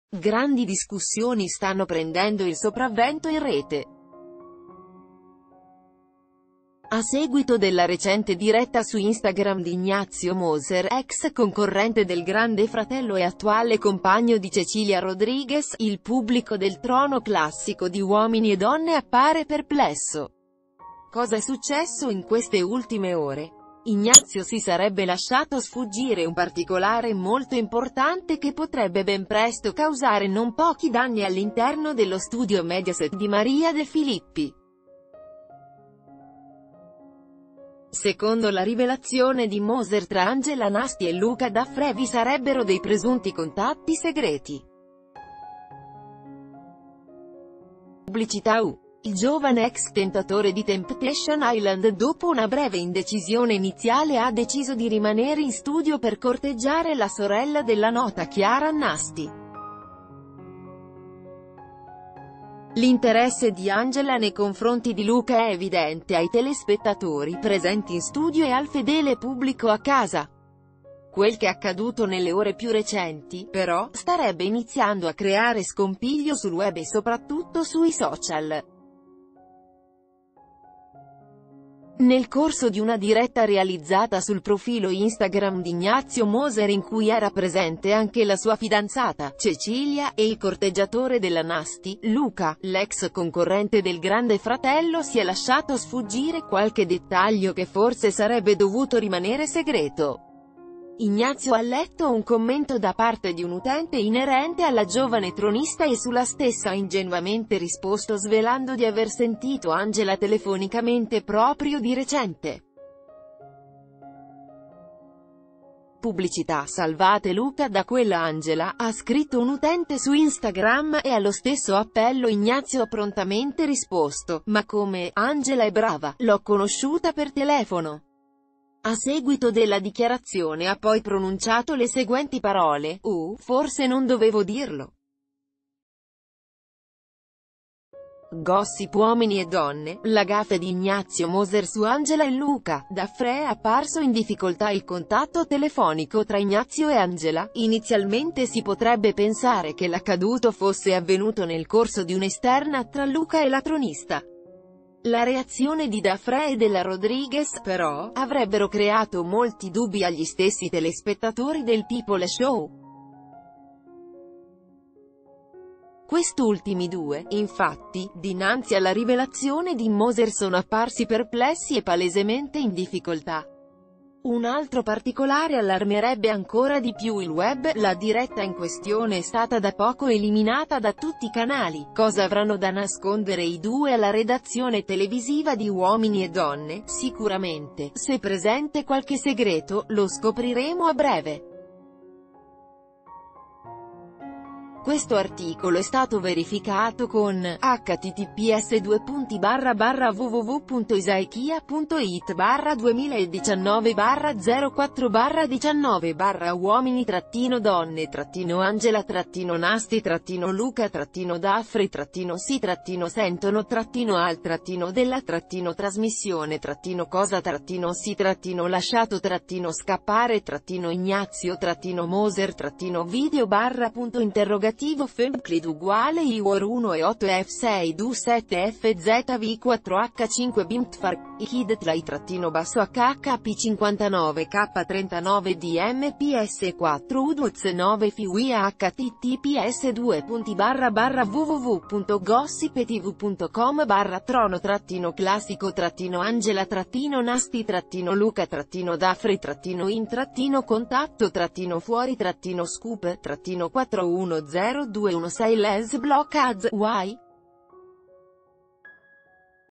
U&D, contatti telefonici forse tra Angela e Daffrè: a rivelarlo la gaffe di Moser su IG
Grandi discussioni stanno prendendo il sopravvento in rete. A seguito della recente diretta su Instagram di Ignazio Moser (ex concorrente del Grande Fratello e attuale compagno di Cecilia Rodriguez), il pubblico del trono classico di Uomini e Donne appare perplesso. Cosa è successo in queste ultime ...